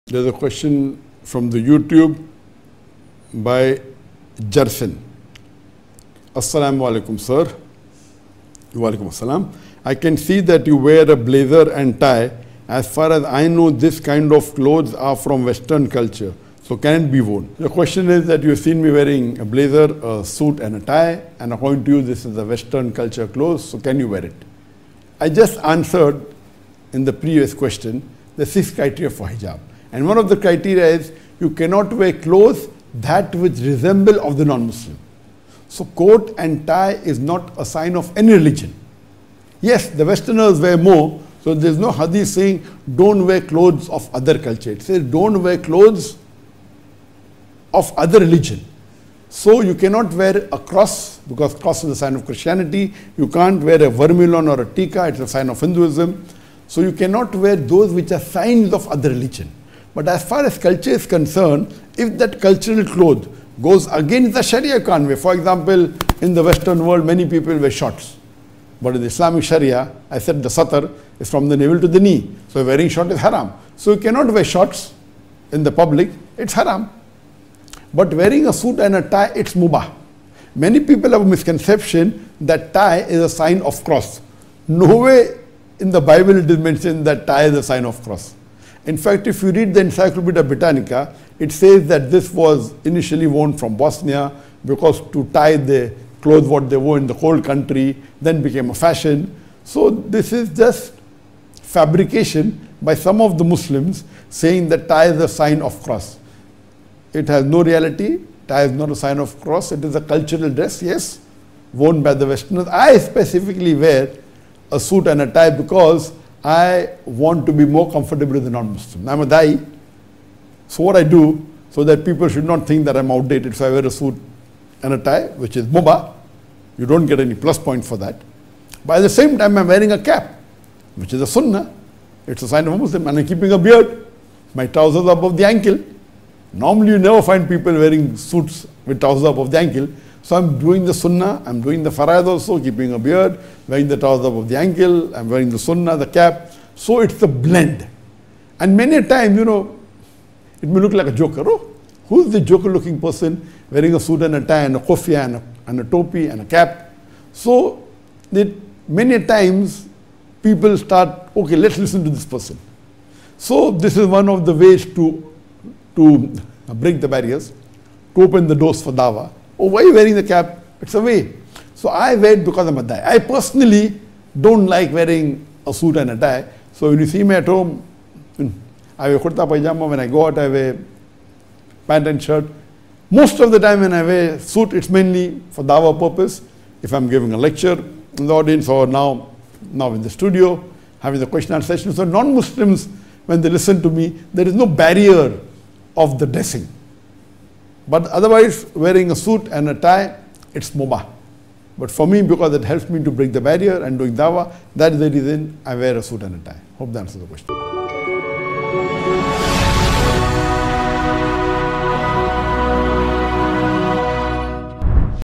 Why do you wear up tie, coat, and blazer it is a Western dress? Question to Dr Zakir Naik
In this video, Dr. Zakir Naik talks about the dress, someone asks Dr. Zakir why did you wore up tie coat and blazer it is a Western dress, in Islam it is correct to wear other cultural dress. He answered this question in the light of Islamic shariyah. He Guides the people to Islam through the authentic sources of the Quran and Sunnah in a contemporary way with the understanding of the righteous predecessors.